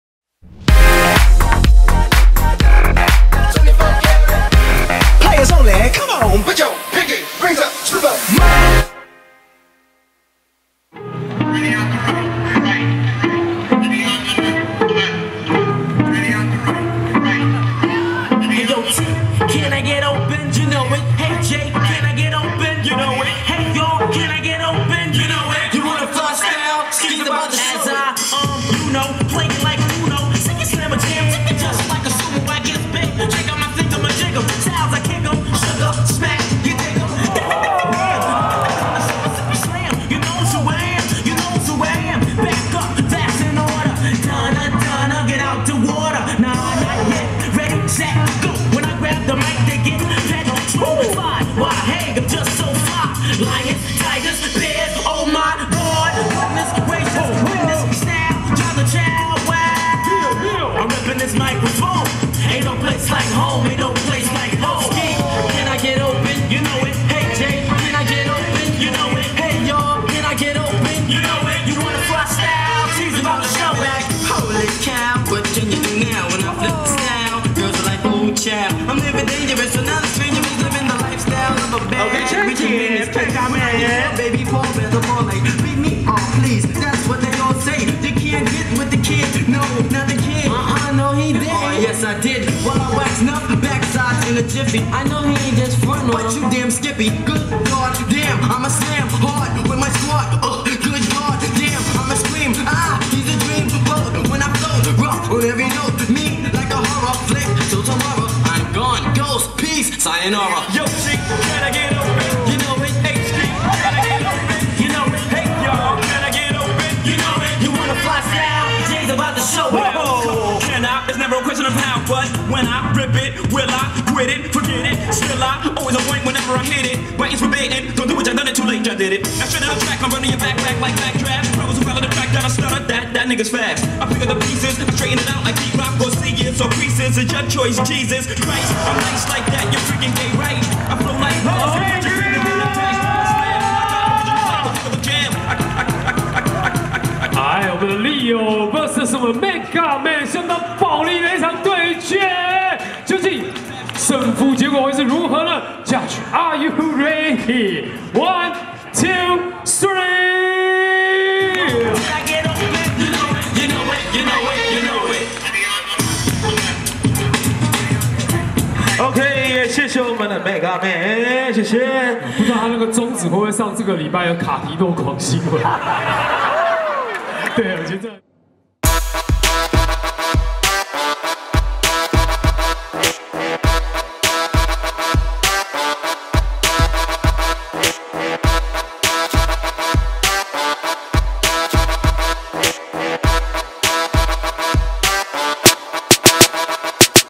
HipHop Best16 5 Mega Man vs Leo | 170228 OBS Vol.11 Day3
Ocean Battle Session vol.11
The best Dance Jam in Taiwan.
An annually international street dance reunion.
No matter how long you have been dancing, we need you here.